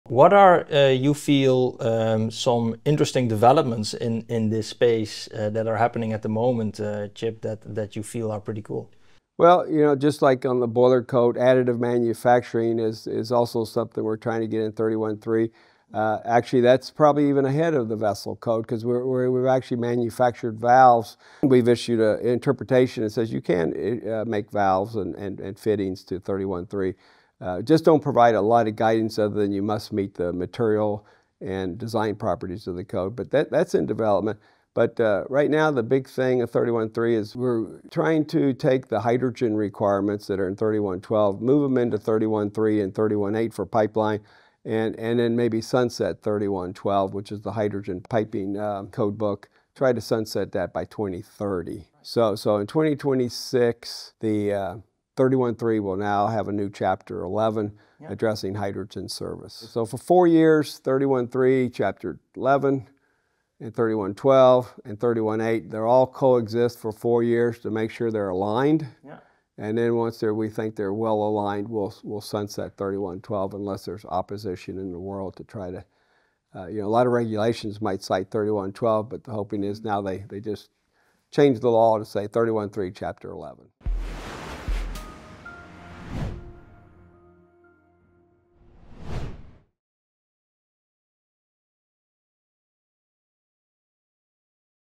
Hydrogen Service & Additive Manufacturing in ASME B31.3 | Interview with Chip Eskridge, PE, CWI
Chip Eskridge, PE, CWI, discusses exciting developments in ASME B31.3 (Process Piping Code) — including the integration of additive manufacturing (3D printing) and the upcoming Chapter 11 on hydrogen service.

Key highlights:

- Additive manufacturing now recognized for valves and fittings under B31.3.
- Guidance for hydrogen piping and pipelines being integrated into B31.3 and B31.8.
- Plans to sunset ASME B31.12 Hydrogen Piping Code by 2030, with Chapter 11 in B31.3 becoming the main reference.

These updates reflect the future of piping engineering, hydrogen infrastructure, and advanced manufacturing.